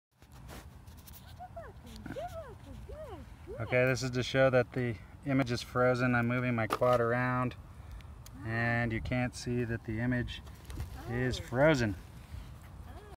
Image frozen on eachine VR D2
Something is wrong with the diversity module and the image freezes after a few seconds of flying.